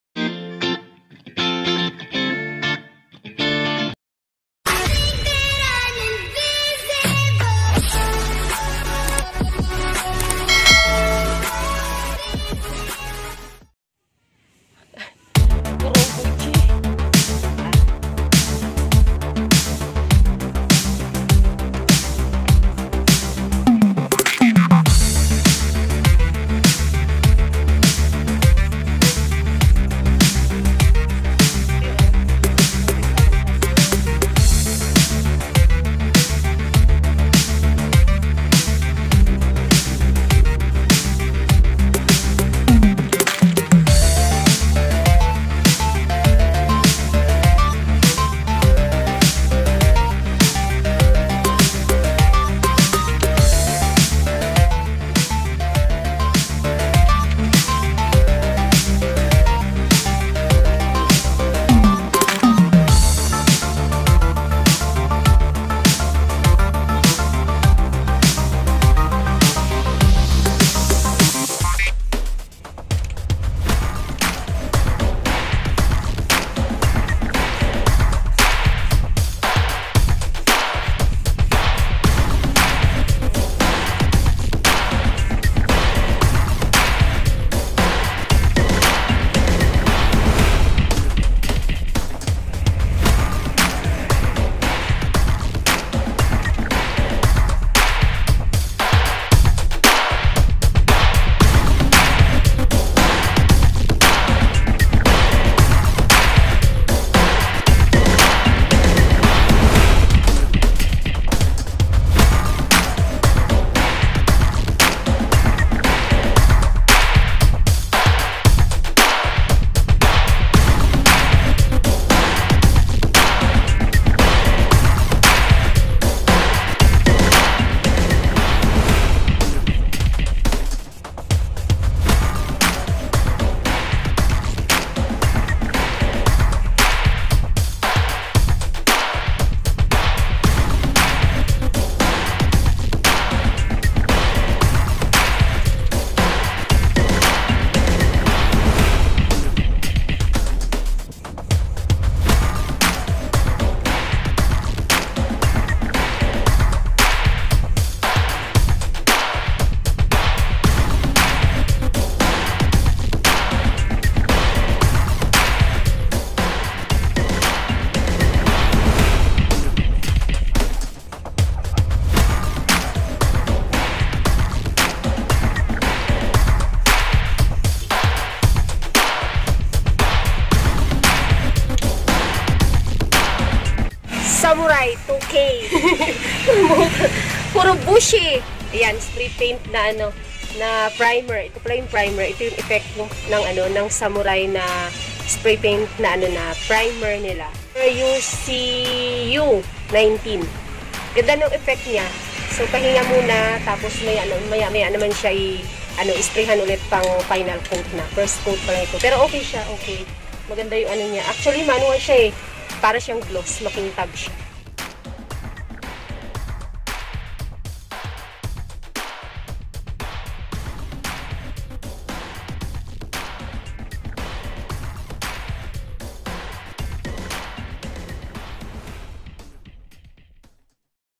GANDA NG SPRAY PAINT NA ITO PANG PRIMER / PRIMER SPRAY PAINT
Hello mga Ka annetrep, Ang primer Ng bosny na madalas binibili Ng mga customers ay grey, pero itong sa samurai is red sya na parang red led na naka pintura.
Pero maganda sya pang primer.